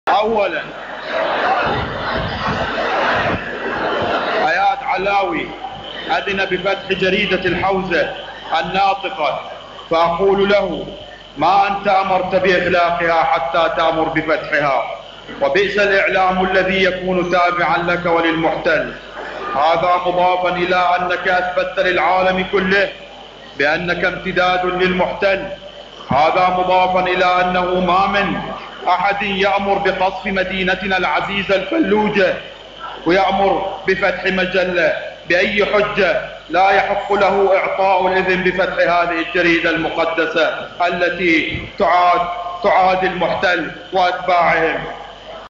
السيد القائد مقتدى الصدر يرد على اياد علاوي بعد امره فتح جريدة الحوزة وقصف الفلوجة
السيد مقتدى_الصدر - الجمعة 67